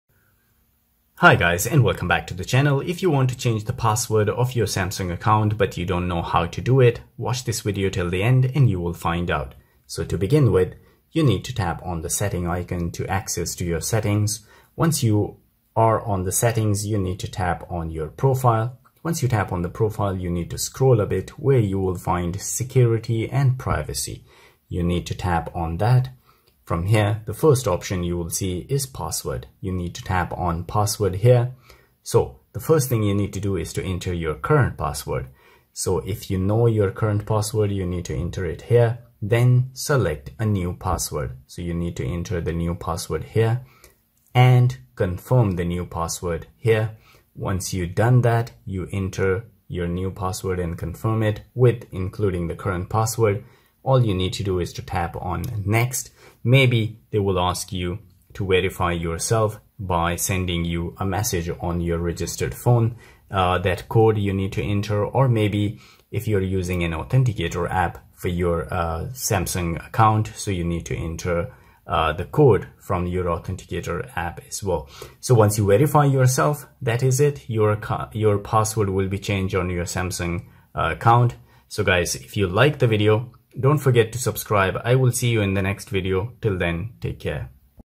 How to Change Password on Samsung Account
Samsung Account, in this video you will learn how to change password on samsung account.